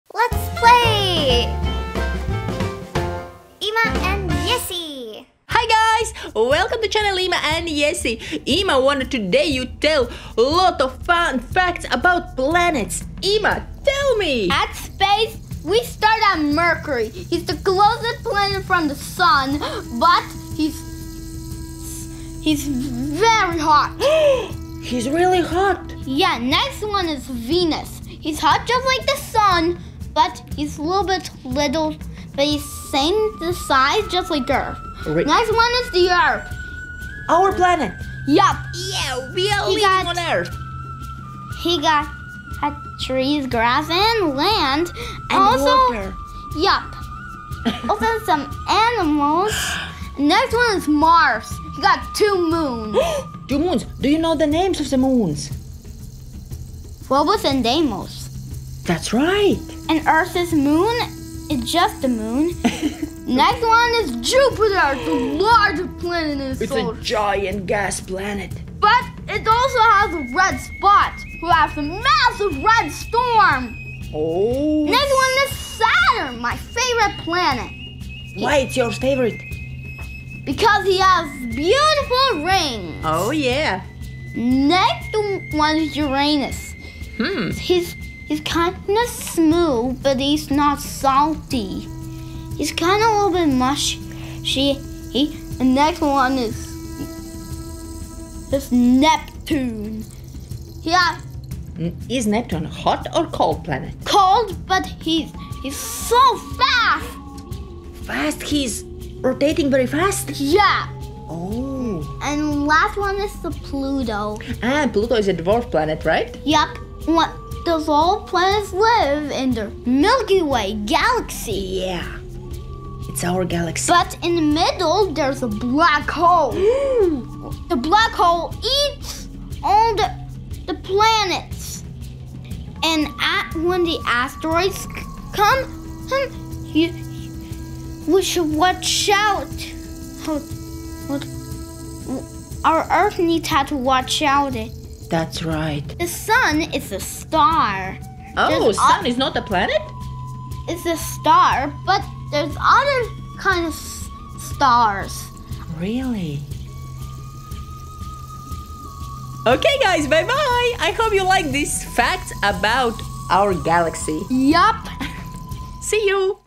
Do you know? Planets and Solar System
Do you know anything about the Solar System and the Planets? Ima will tell you more about Earth, Mars, Jupiter, Mercury, Saturn, Uranus, Pluto, Venus, Moon, Sun  and even about the Black Hole.

Visit the Ima and Jessy channel as often as you like and watch educational and funny videos for children every day.